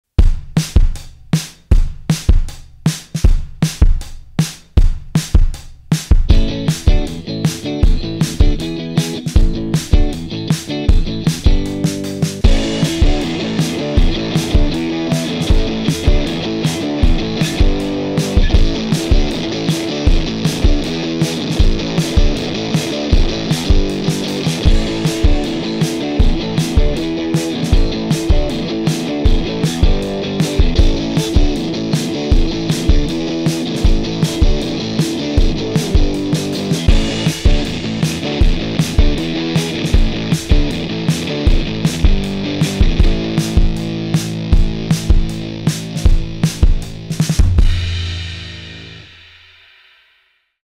5 Distortion Pedals vs UAFX Ruby 63
In this video I‘m testing the UAFX as a platform for different distortion type pedals.

The Guitar: Fender #95 Made In Japan Jazzmaster with Lollar J-Master Set PUs (more on the guitar
The Pedals: ProCo Rat, JHS 3 Series Fuzz, Boss SD-1, Boss DS-1w, EHX Mainframe